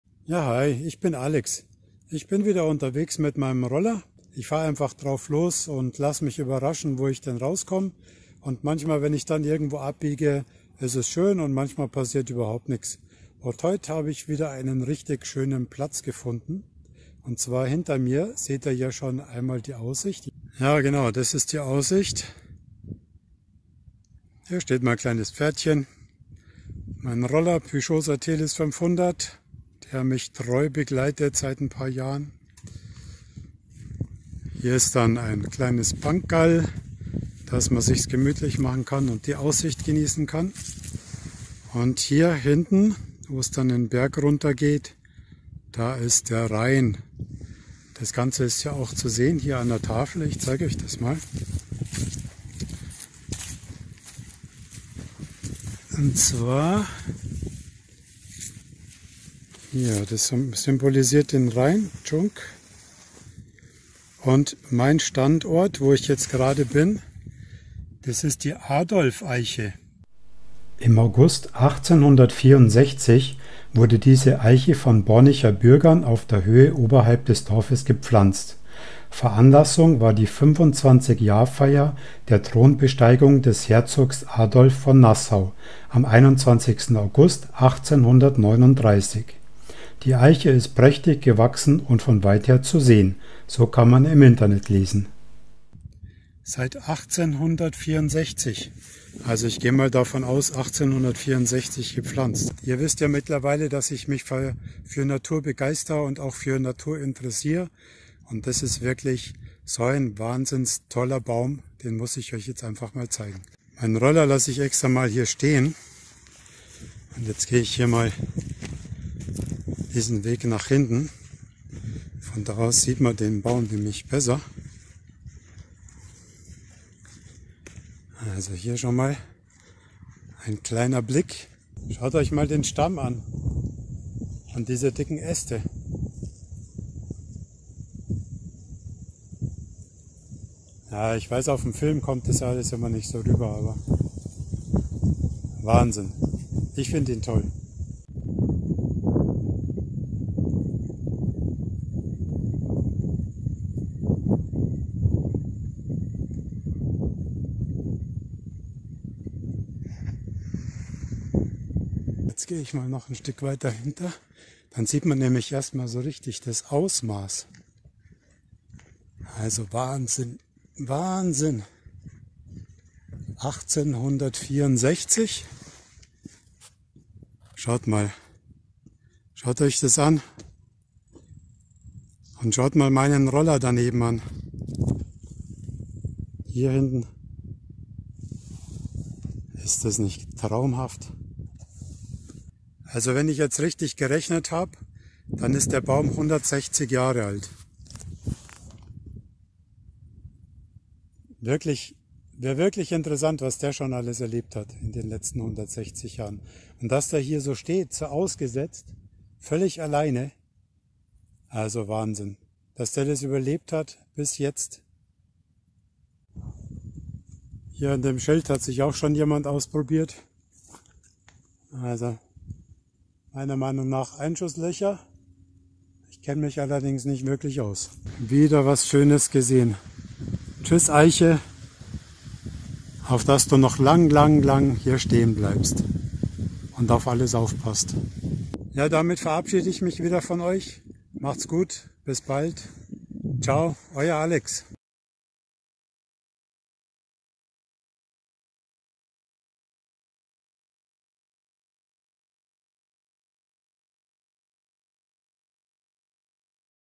160 Jahre alte Eiche, 1864 von Bornicher Bürger gepflanzt zur Thronbesteigung des Herzogs von Nassau
Im August 1864 wurde diese Eiche von Bornicher Bürger auf der Höhe oberhalb des Dorfes gepflanzt.
Veranlassung war die 25-Jahr-Feier der Thronbesteigung des Herzogs Adolph von Nassau am 21.August 1839. Die Eiche ist prächtig gewachsen und von weither zu sehen. So im Internet zu lesen.